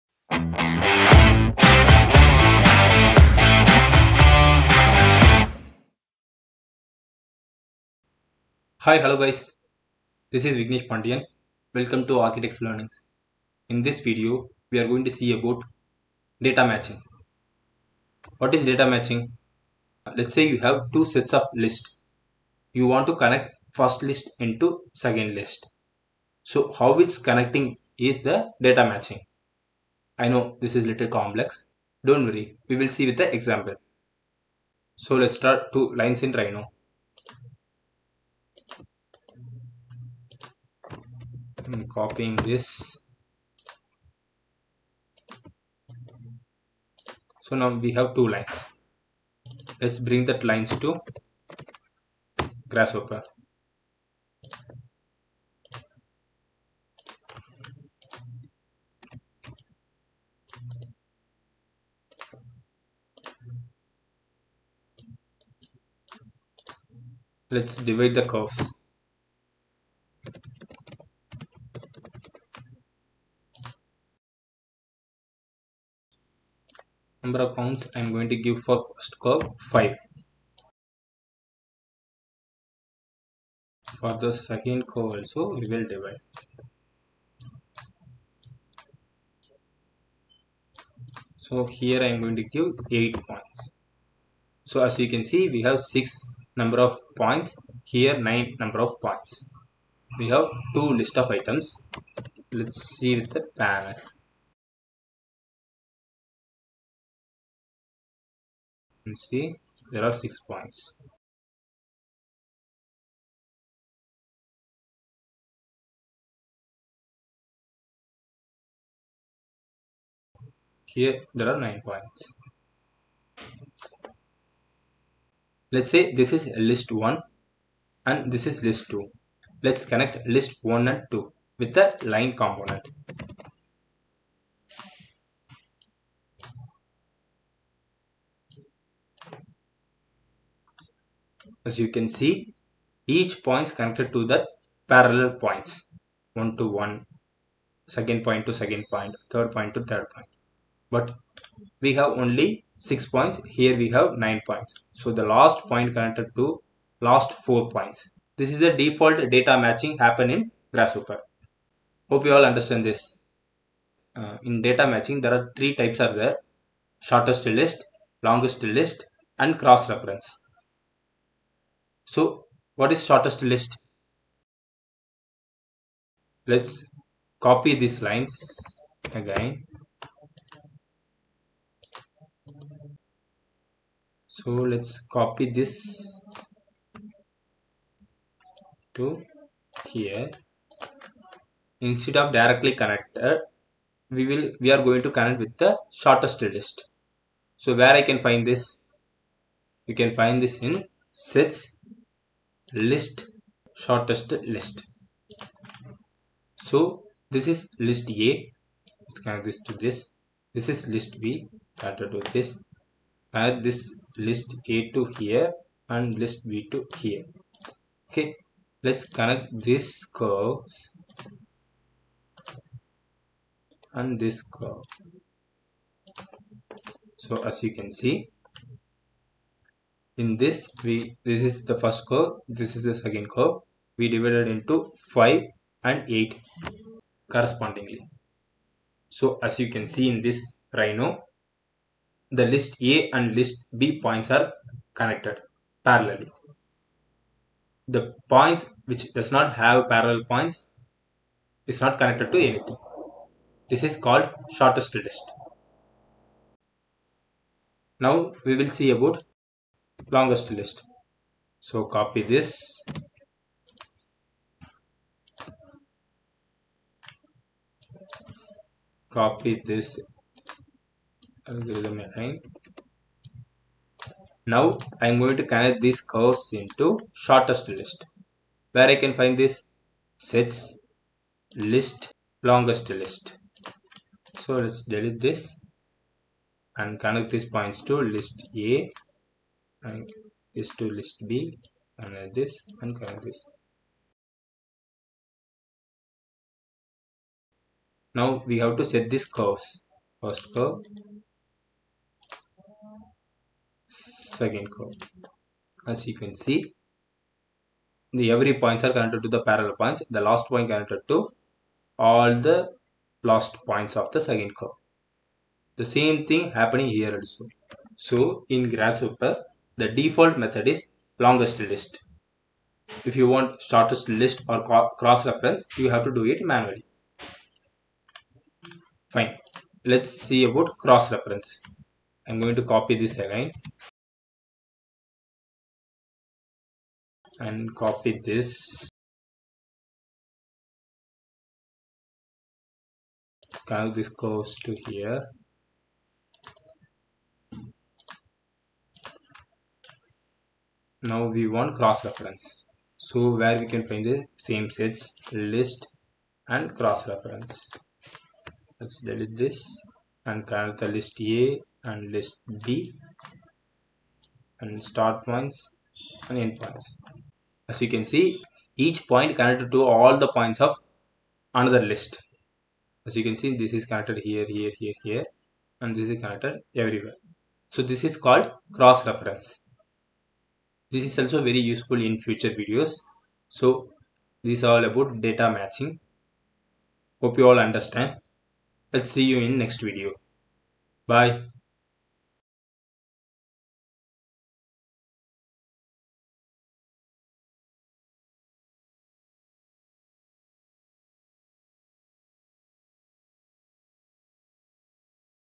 Different ways to connect points in grasshopper? _ Beginner tutorial | Data Matching| EP- 17
This is my 17 th component video of the Rhino-Grasshopper Tutorial.
We will take one component and see how to use it.
In this video, we are seeing the different ways to connect points in Grasshopper.

Tutorial Reference
AAD_Algorithms-Aided Design
Parametric Strategies Using Grasshopper®
Author: Arturo Tedeschi
Foreword: Fulvio Wirz (Zaha Hadid Architects)